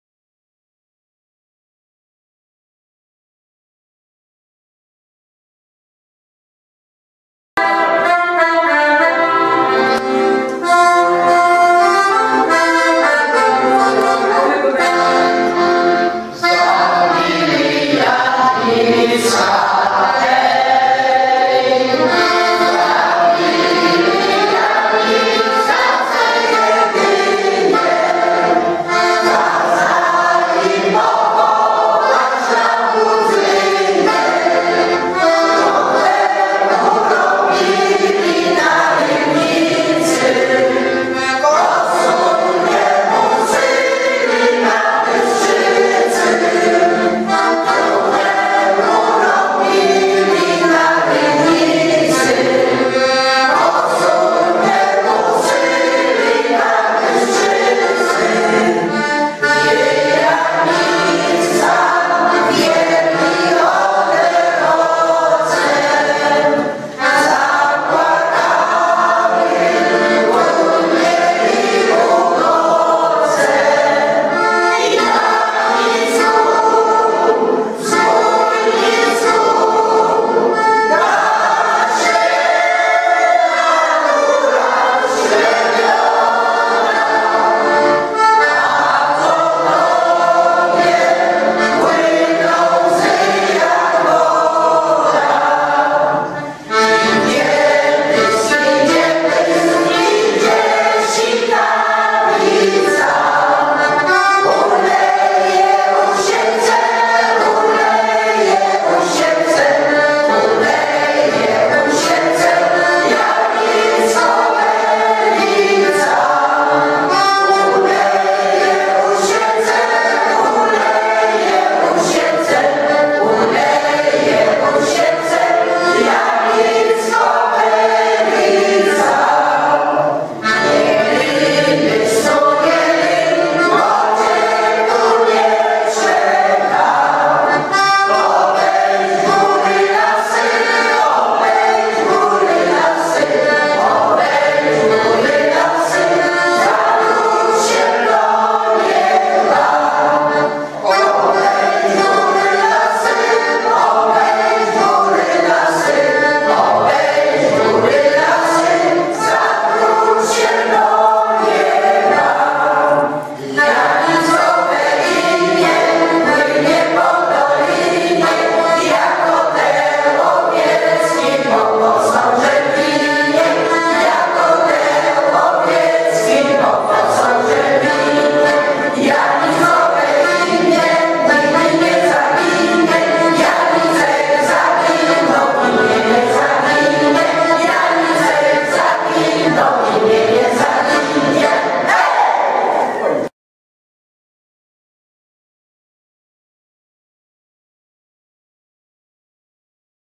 Próba przed występami Chóru Podgórski Walczyk z Torunia [wideo]
W dniu 19 stycznia 2015 r. w Domu Muz na ul. Poznańskiej 52 w Toruniu odbyła się próba chóru przed występami.
Prowadzącym chór jest Józef Hołubek.
Kamera - Jacek Mroczek
Studio Filmowe - KREATYWNY SENIOR